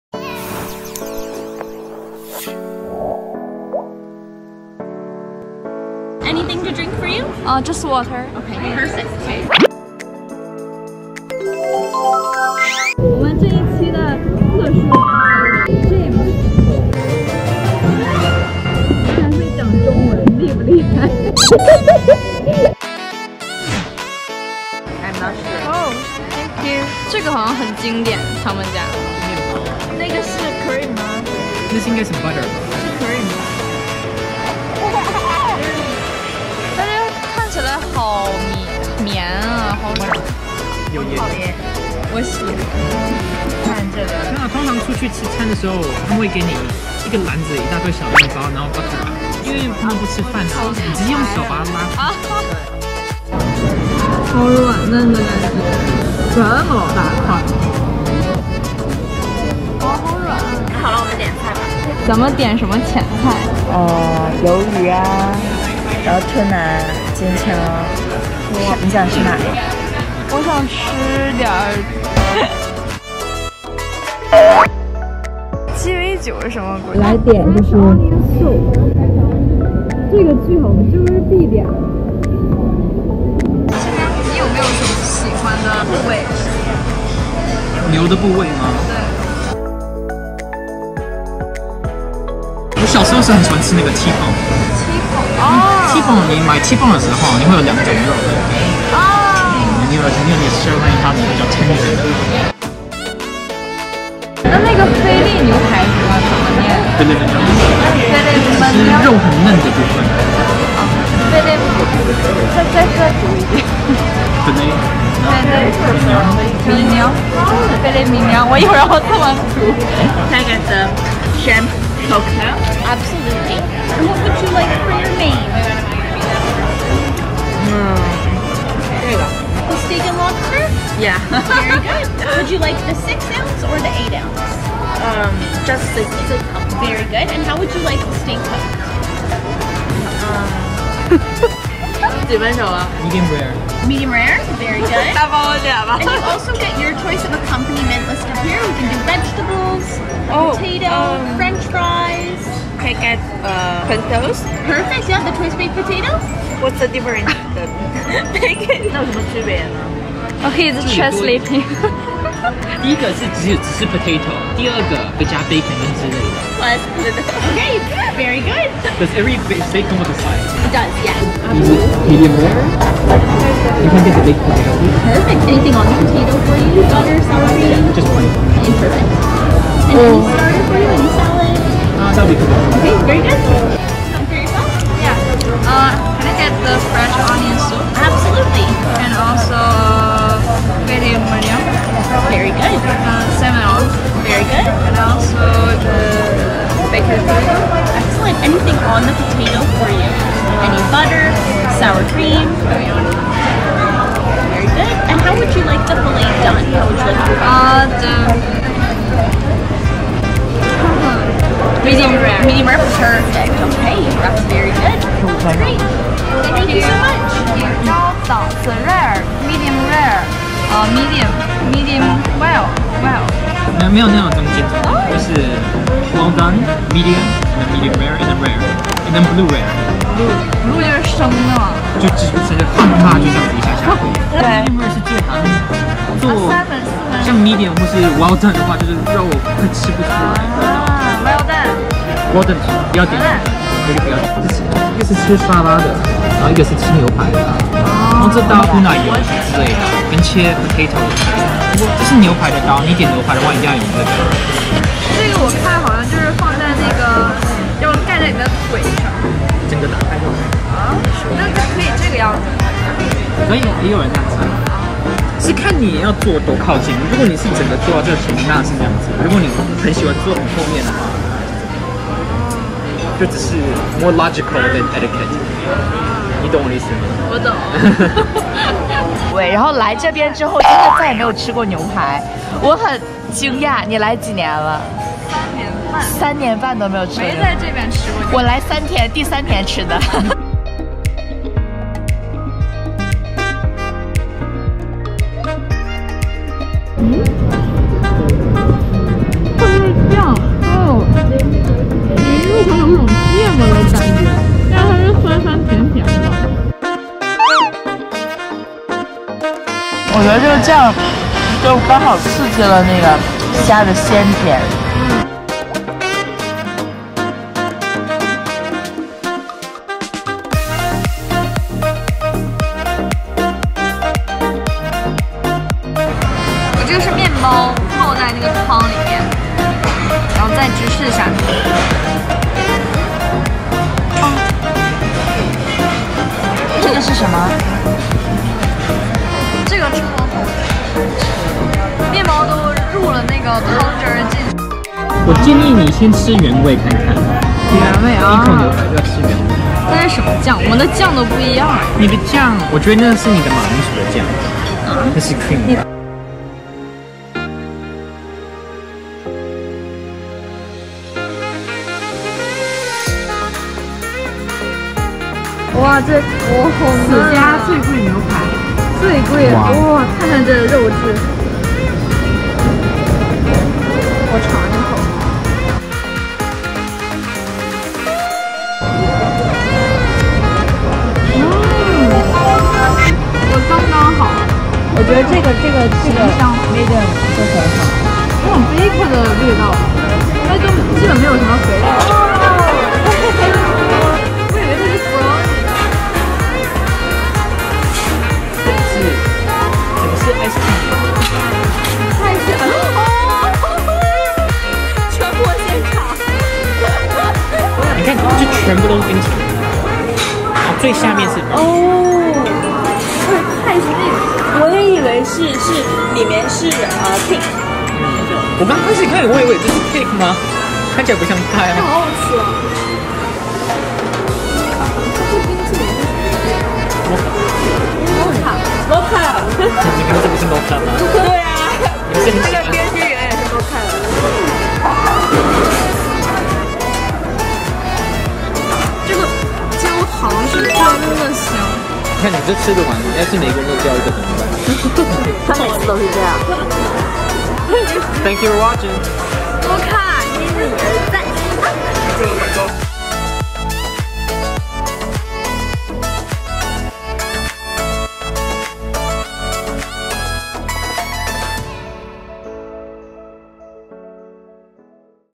Chinese try Popular Western Restaurant混血小哥教你如何在西餐厅点餐｜英文点餐
探店名称name：The Keg Steakhouse+Bar
探店地址address： 2201 Yonge Street
详细图文测评在微信公众号：MOCA多伦多探店